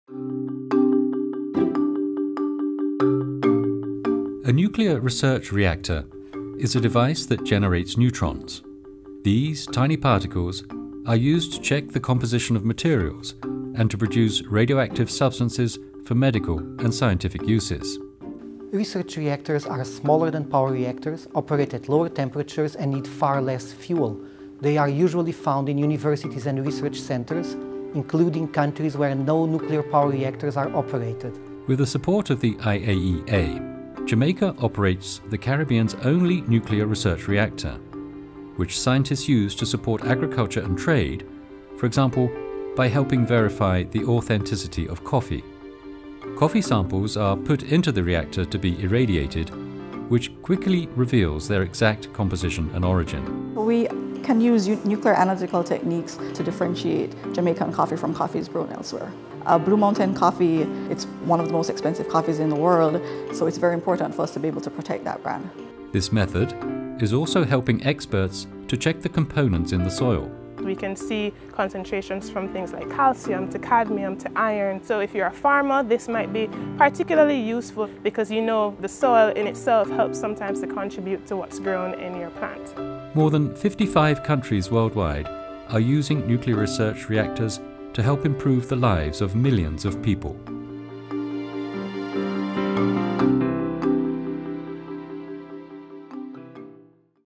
What is a Nuclear Research Reactor?
Nuclear research reactors generate neutrons used to produce radioactive substances for medical and scientific uses and to check the composition of materials. With the support of the IAEA, Jamaica operates the Caribbean’s only nuclear research reactor.

Story: Alejandra Silva
Video: Svetlomir Slavchev
Narration: John Brittain